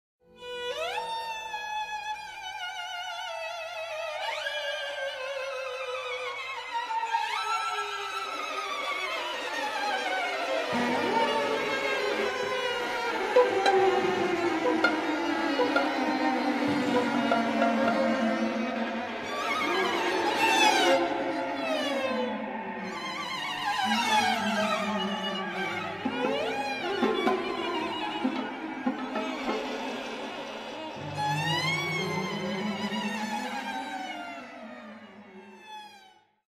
Paper Model of the Red Door from the "Insidious" Movies
This took me about an hour and a half to make. PLEASE COMMENT!